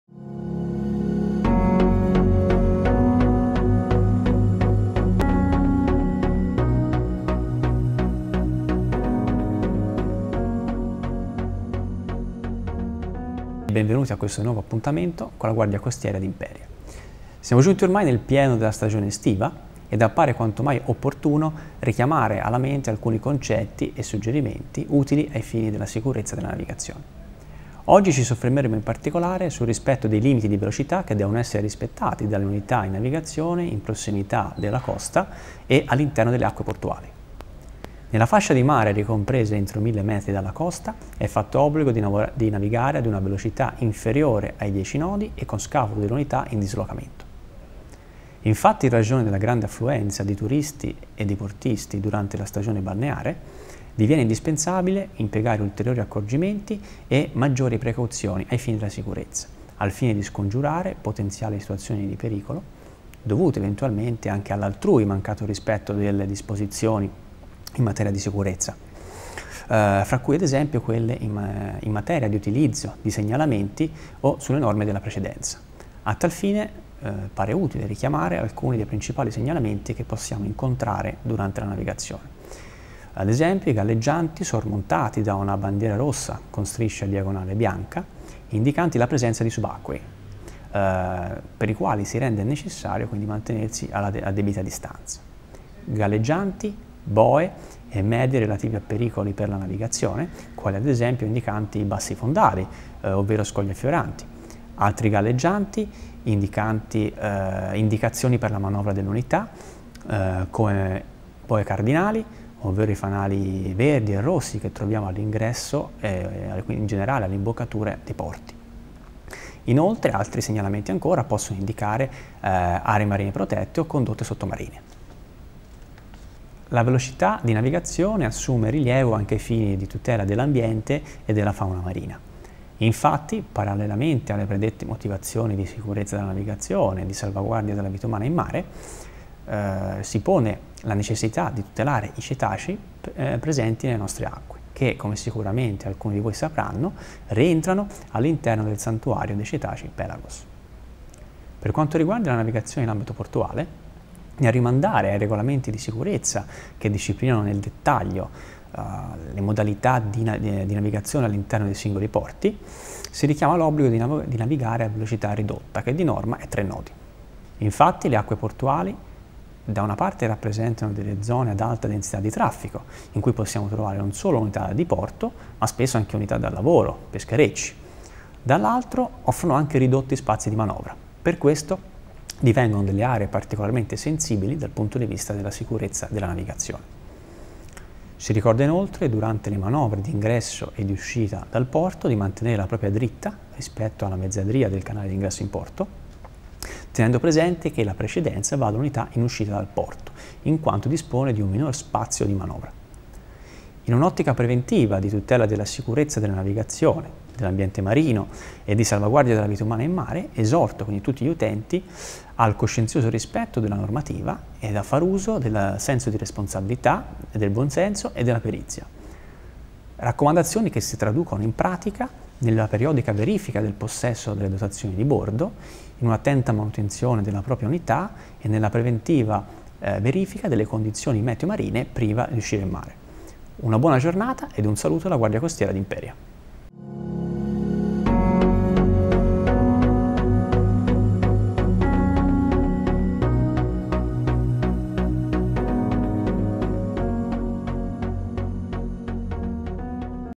MARE SICURO 2018 : I CONSIGLI DELLA CAPITANERIA DI PORTO SU IMPERIA TV
Anche quest’anno Imperia TV dedica uno spazio all’operazione Mare Sicuro, in collaborazione con la Capitaneria di Porto di Imperia